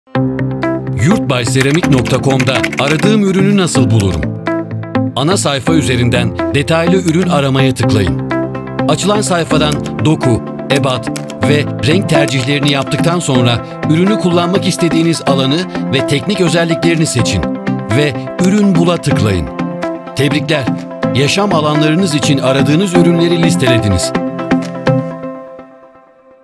Yurtbayseramik.com'da Aradığım Ürünü Nasıl Bulurum?
Yurtbayseramik.com'da tercih ettiğiniz ürünleri "Detaylı Ürün Arama" ile birkaç adımda kolayca ve hızlıca listeleyebilirsiniz. Detaylar videomuzda.

Doğadan Sanata